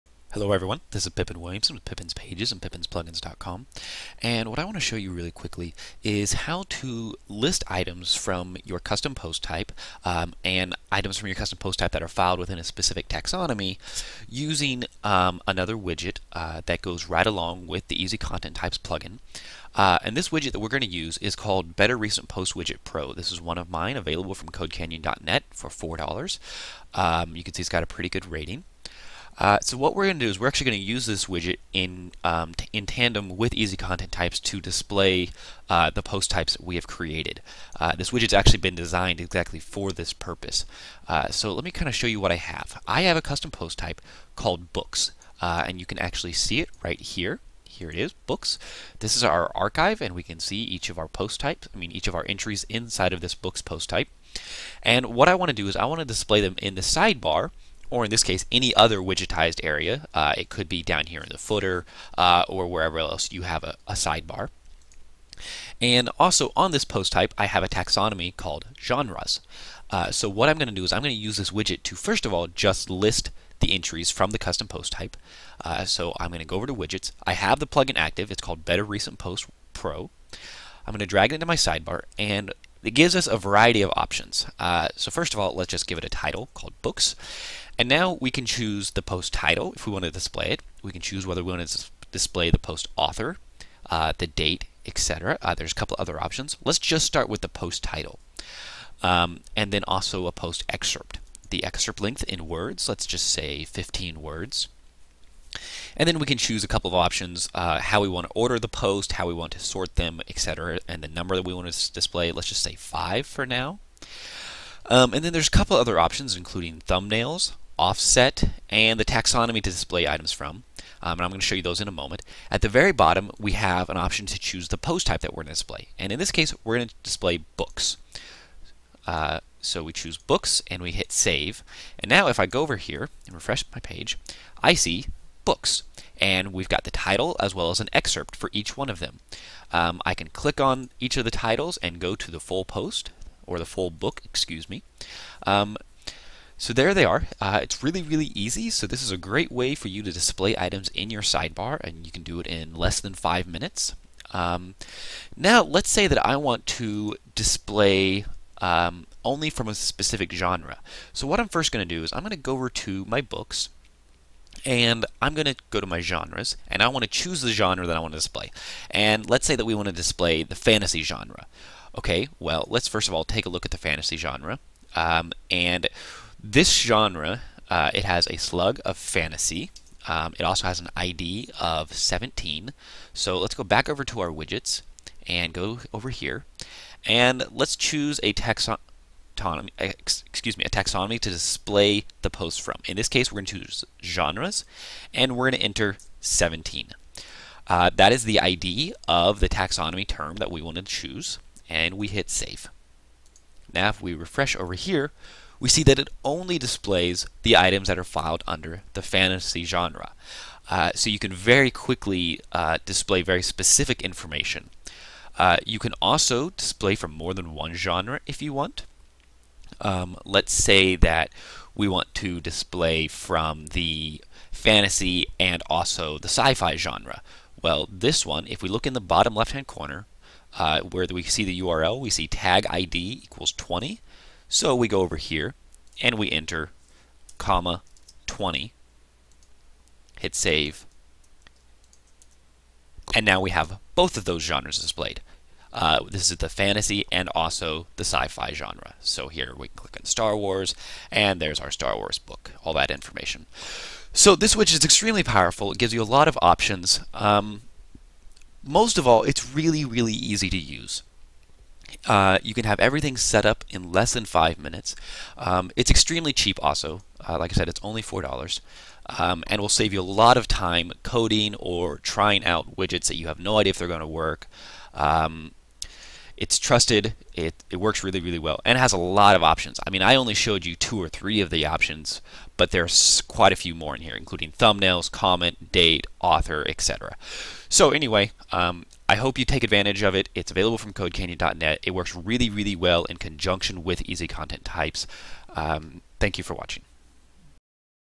How To Display Custom Post Types In Widgetized Sidebars
This tutorial demonstrates how to display a list of recent custom post type entries in any widgetized sidebar in less than 5 minutes.

The widget is provided Better Recent Posts Widget Pro plugin, and it is exceptionally powerful, yet very easy to use. The widget will allow us to list entries from any custom post type, and also give us the option of restricting the entries displayed to those filed in a specific custom taxonomy term.